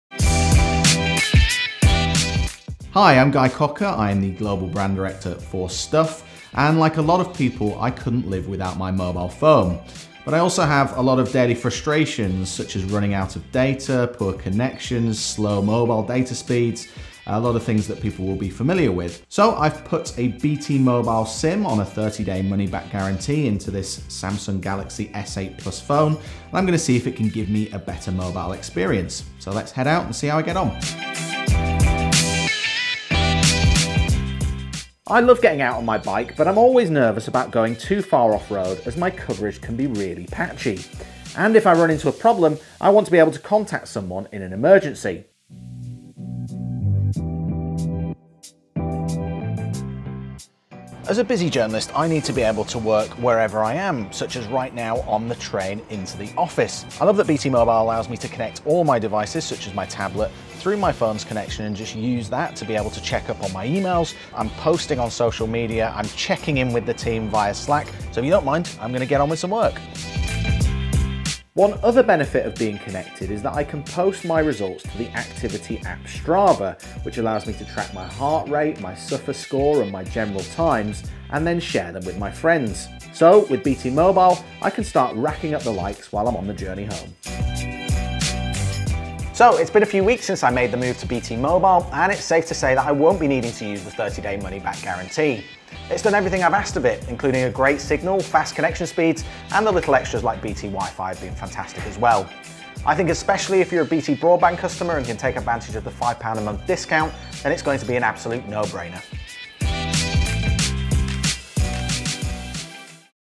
Should you switch to BT Mobile
A promoted video for BT Mobile and Stuff.tv. Shot on the Arri Alexa Mini and Sony FS5 in 4K raw. Shot in a hired location in Twickenham, London.